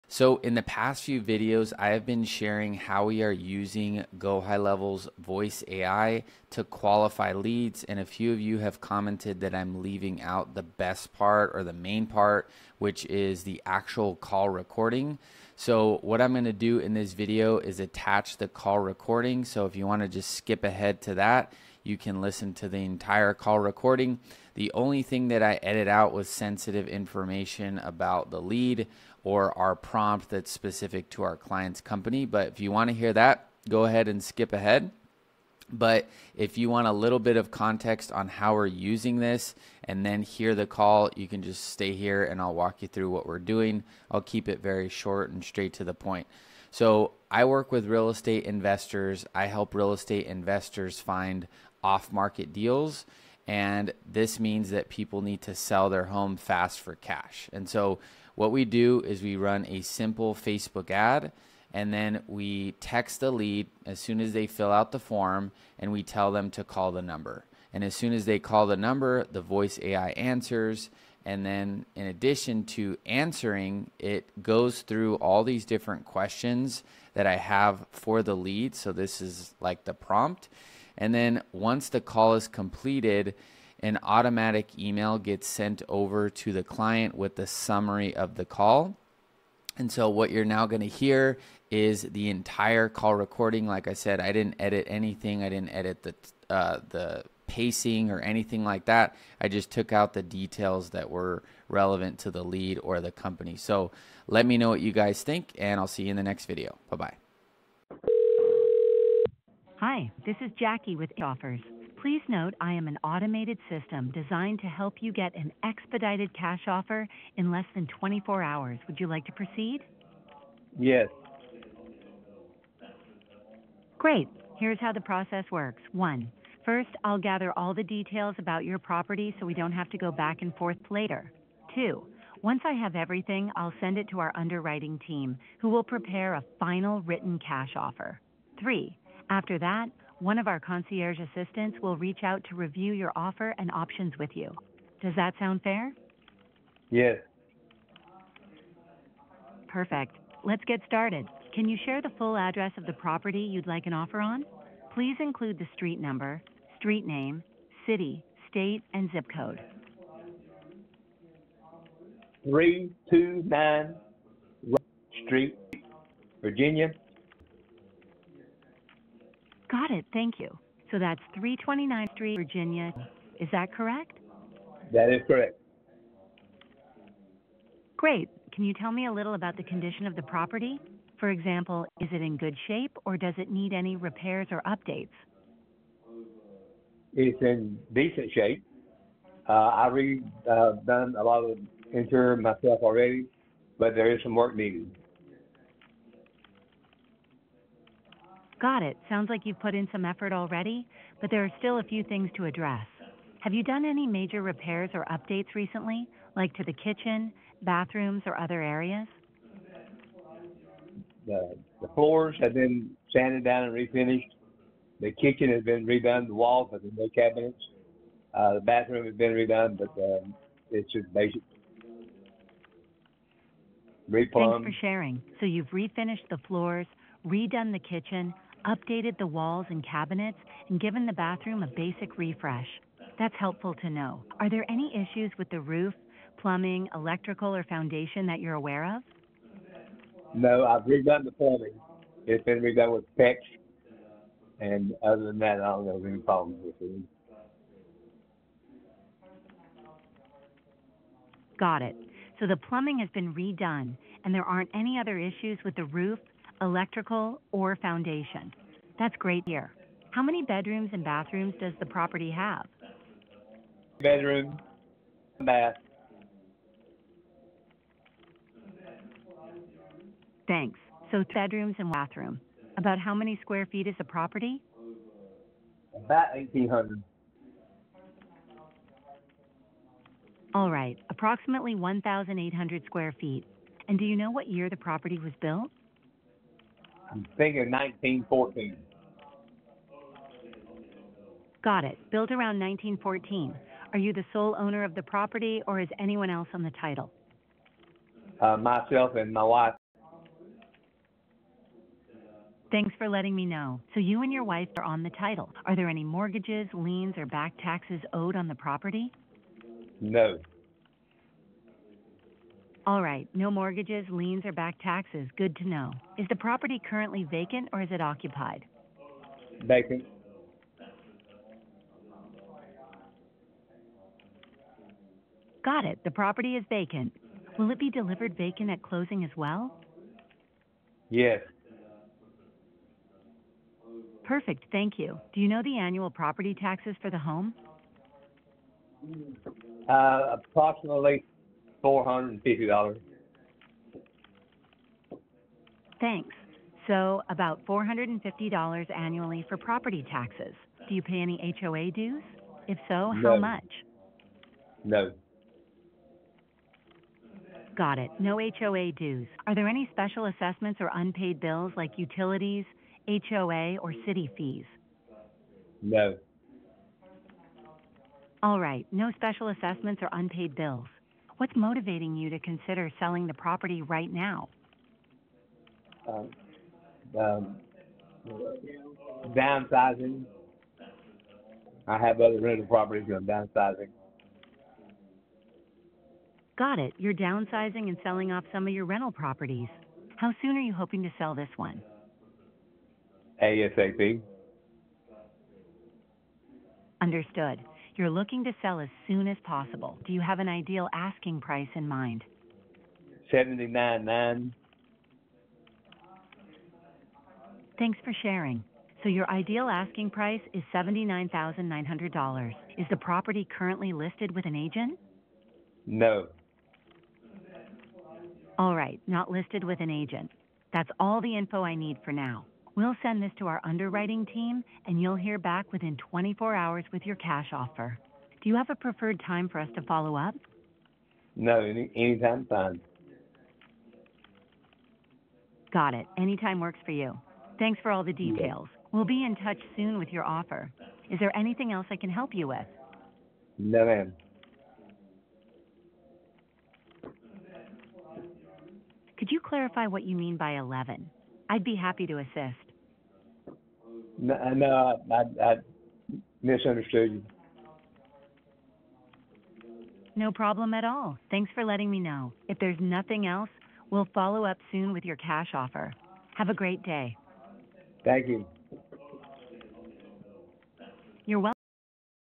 GoHighLevel Voice AI Qualifies Lead (Live Call Recording)
Get access to 30 day trial to HighLevel + all my EXCLUSIVE bonuses ($97, $297):